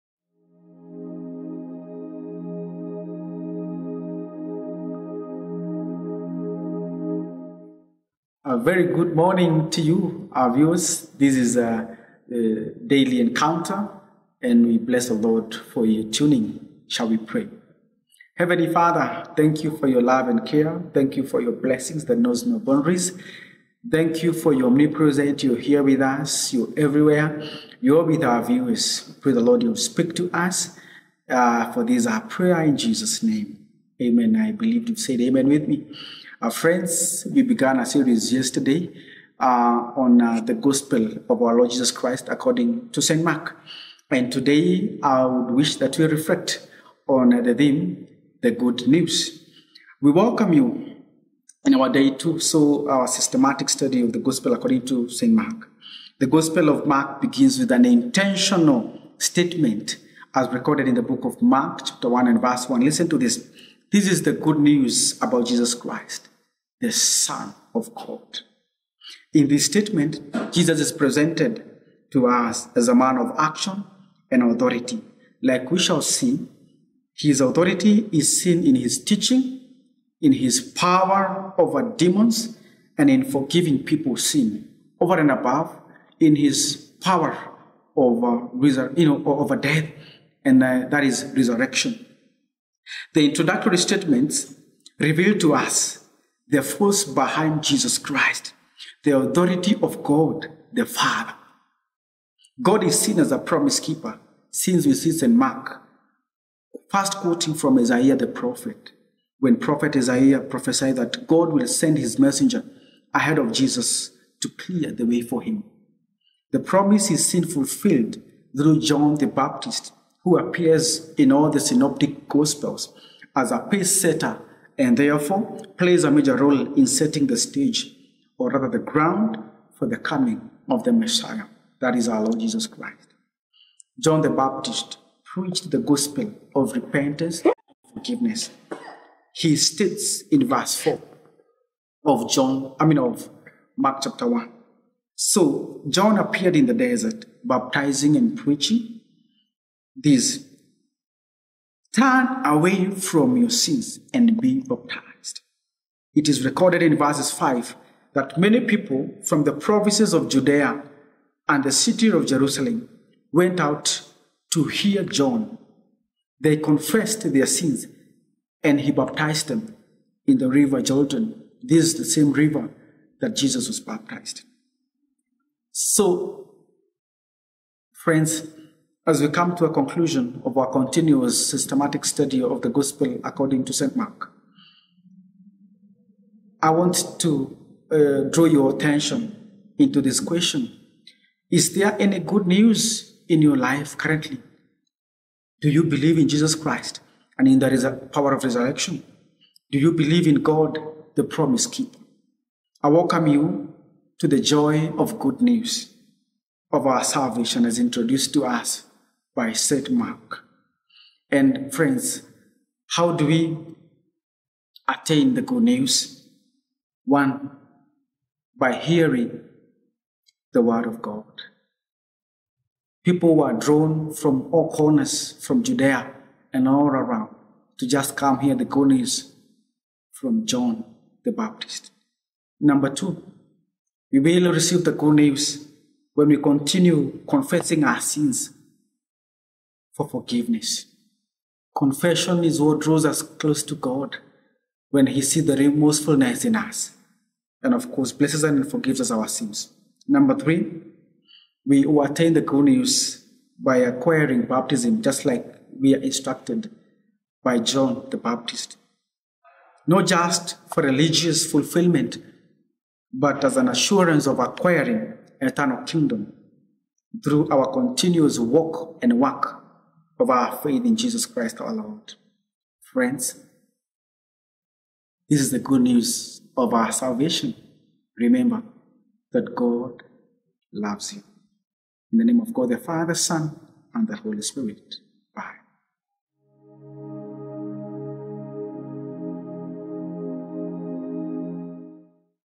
YOUR DAILY ENCOUNTER The gospel according to St Mark Day 2 week 1 Theme: The Good News
The gospel according to St Mark

Day 2 week 1

Theme: The Good News

We welcome you in our day 2 of the systematic sturdy of the gospel according to St. Mark.
The Gospel of Mark begins with an intentional statement “This is the Good News about Jesus Christ, the son of God” Mark 1:1
In this statement Jesus is presented to us as a man of action and authority. Like we shall see, His authority is seen in His teaching, in His power over demons and in forgiving people’s sin.

The introductory statement reveals to us the force behind Jesus Christ. The authority of God the father.
God is seen as a promise keeper since we see St. Mark first quoting from Isaiah the prophet when Prophet Isaiah prophesied that God will send His messenger ahead of Jesus to clear the way for him.

The promise is seen fulfilled through John the Baptist who appears in all the synoptic gospels as a pace setter and therefore plays a major role in setting the stage/ ground for the coming of the messiah.
John the Baptist preached the gospel of repentance and forgiveness. He states in verse 4, “So John appeared in the desert, baptizing and preaching. “Turn away from your sins and be baptized,” It is recorded in verses 5 that many people from the province of Judea and the City of Jerusalem went out to hear John. They confessed their sins and he baptized them in the River Jordan.

As I come to a conclusion, is there any good news in your life currently? Do you believe in Jesus Christ and in the of Resurrection? Do you believe in God the promise keeper?
I welcome you to the joy of Good news of our salvation as introduced to us by St. Mark by

I. Hearing the word of God

ii Confessing your sins for forgiveness

iii Acquiring baptism not just for religious fulfilment but as an assurance of acquiring eternal kingdom through our continual walk and work of our faith in Jesus Christ our Lord.

AMEN